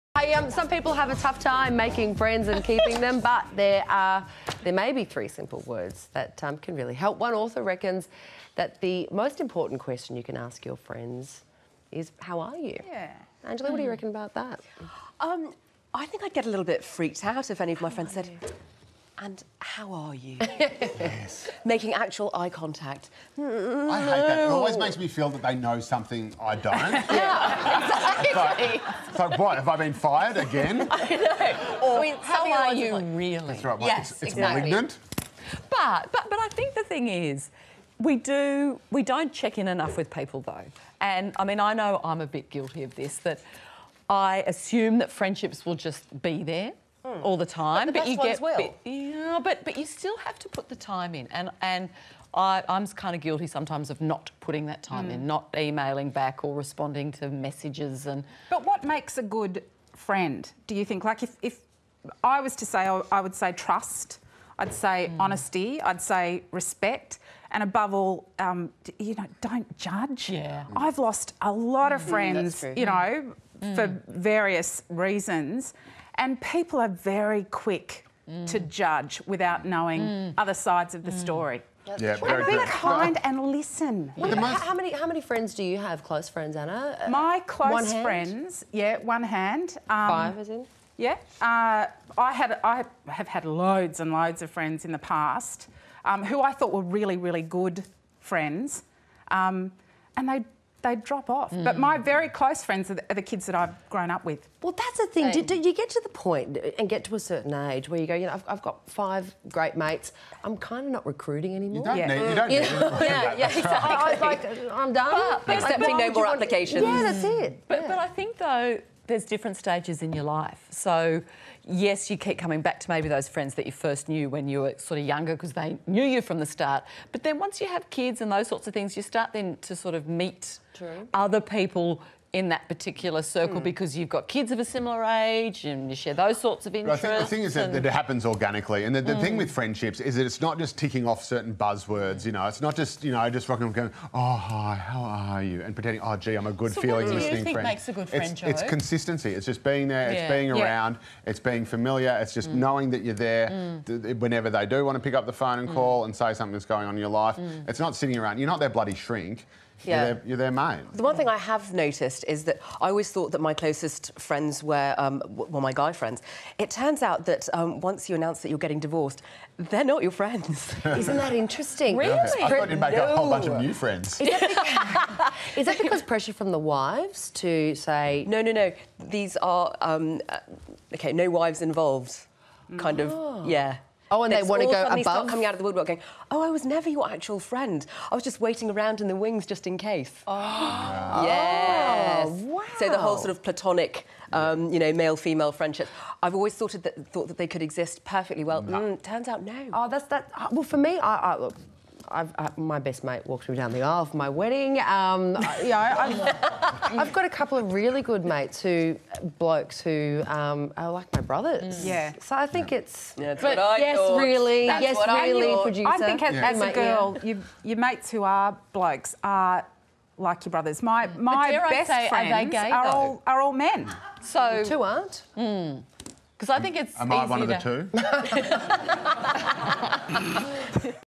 How To Win Friends
Some people have a tough time making friends and keeping them - but there may be three simple words that really help.